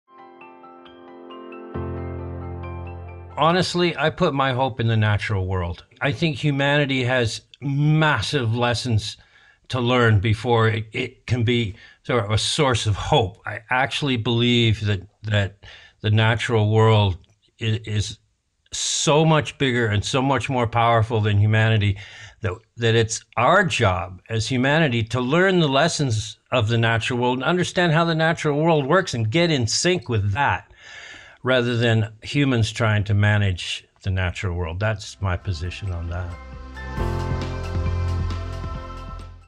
What gives you hope?
Rex Weyler talks about where he finds hope in today’s world and the massive lessons that humanity need to learn from the natural world.